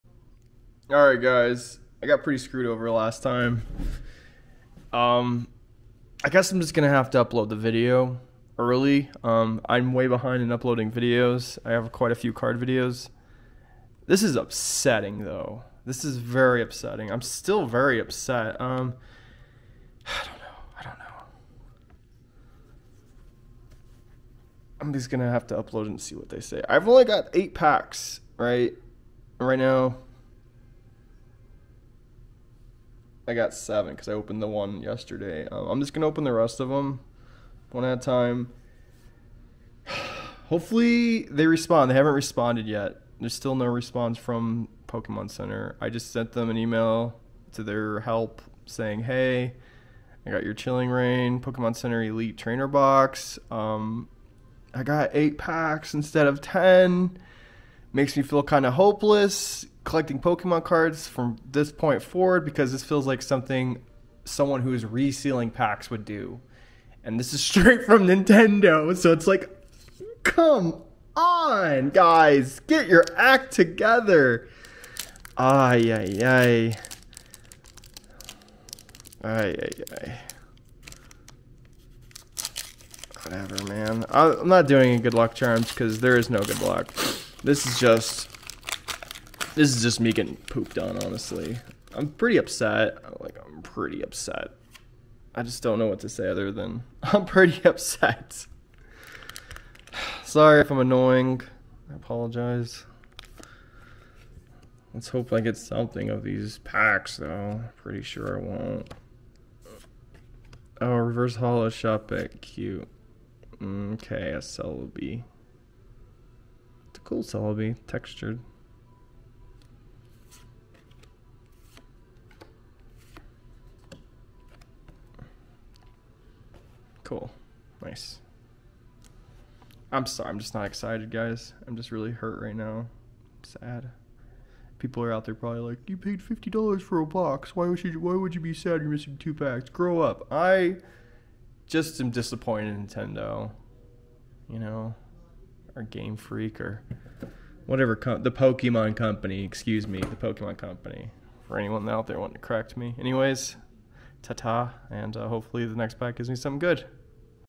Pokemon Center ETB Chilling Reign Opening Pack #2 + me complaining the whole time.
I explained the reason I am complaining in a video. :( still sad they messed up but they are making up for it.